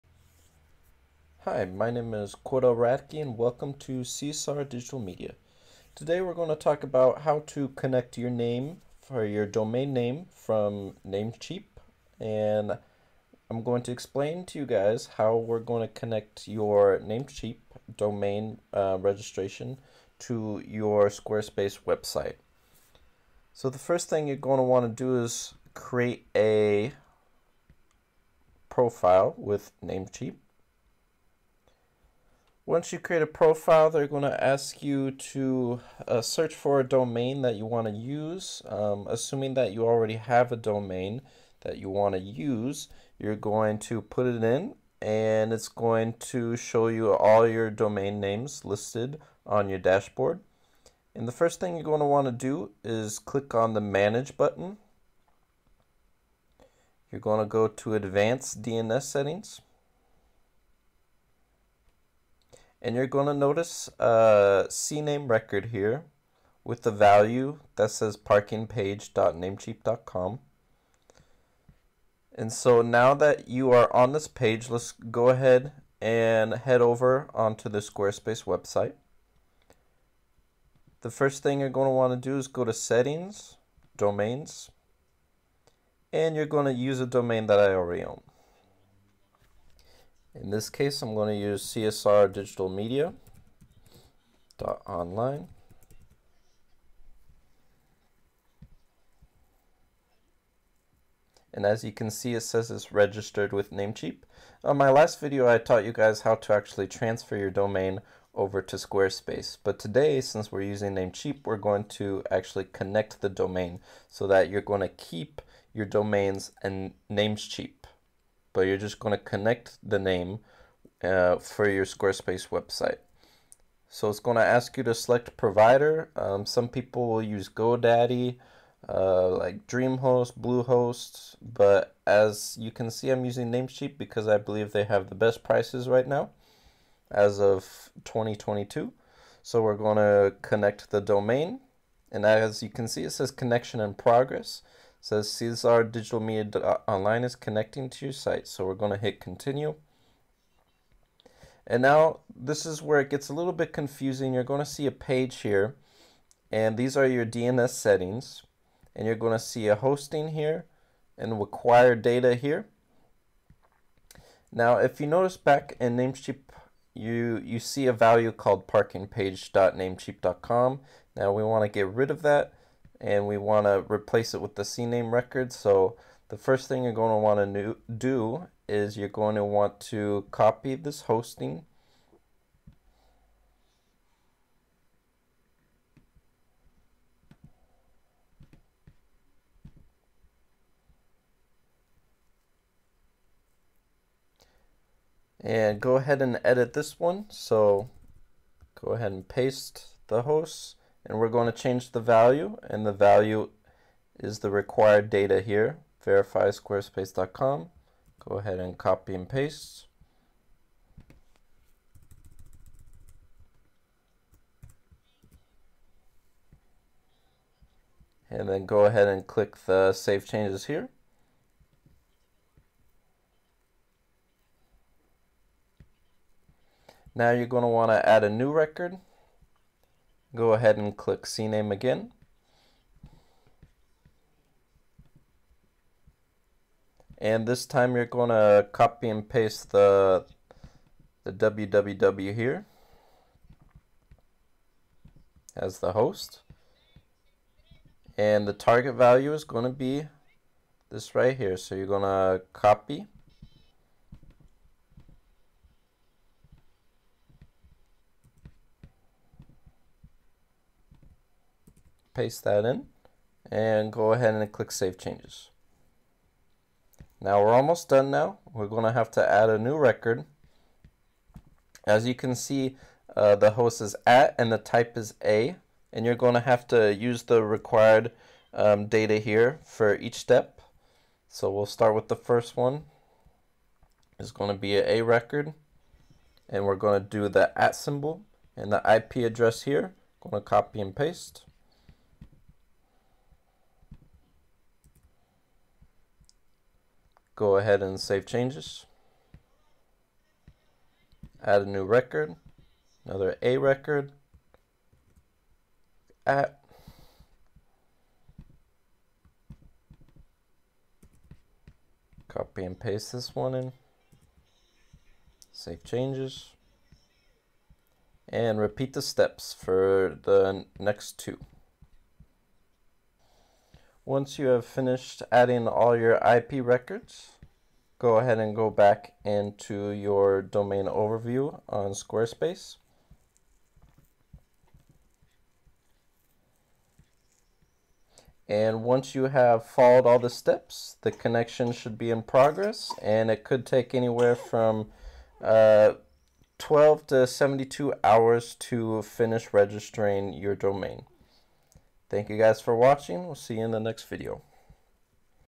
How to Connect your Namecheap Domain to Squarespace
Made this video very quick for those who were asking how to set up their domains with Squarespace. I may make more professional and in-depth tutorial videos if I get enough likes and subscribers.